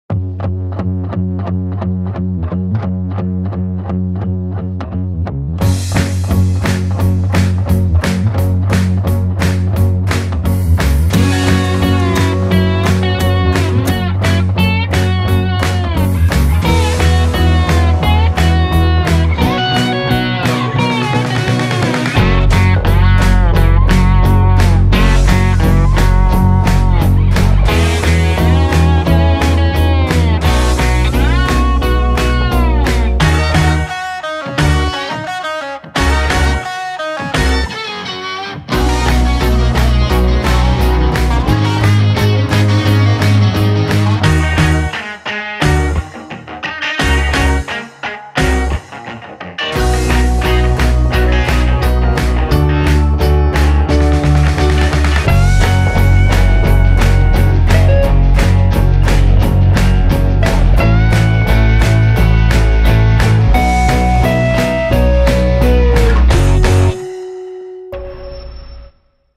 promo Yamaha Tricity 300 [new model for 2020]
Promo demo Το νέο Τricity300 έρχεται μέσα στο πρώτο τρίμηνο του 2020 και θα οδηγείται με δίπλωμα αυτοκίνητου. Με απίστευτο πρωτοποριακό μπροστινό σύστημα σχεδόν όπως το Νιkken μπορεί να σταθεί ακινητοποιημένο χωρίς να χρειάζεται να κατεβάσετε τα ποδιά σας. Ο κινητήρας είναι από Χmax300 με κατανάλωση 3,5lit/100km
ADP Productions - Logo Design by Phil Delves